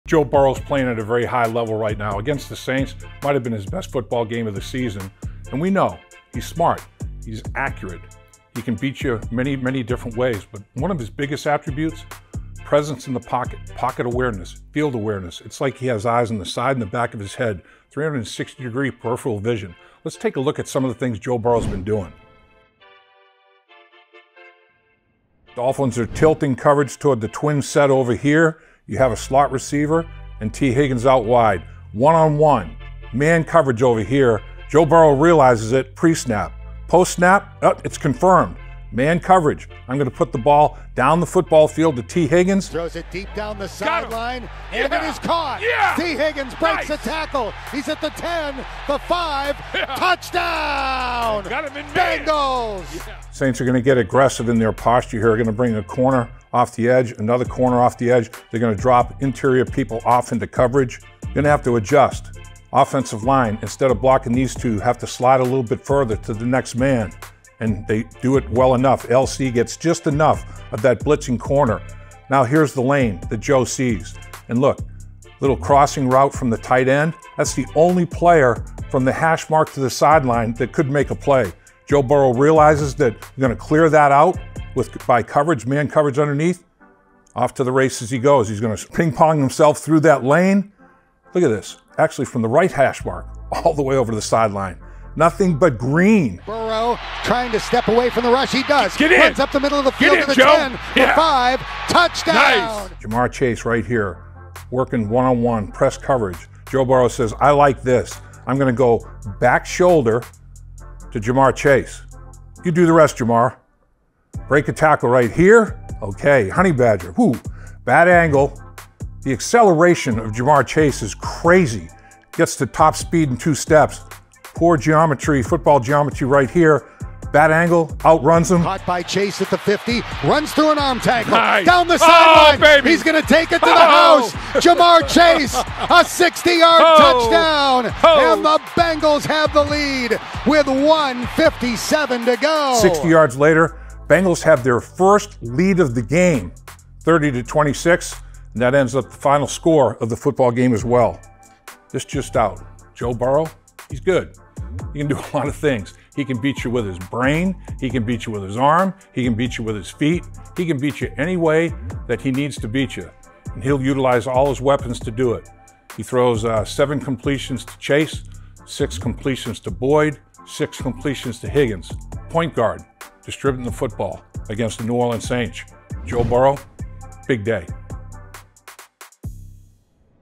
Joe Burrow's Impact on the Offense | Lap's Look
Dial in on Dave Lapham as he breaks down Joe Burrow's best plays this season.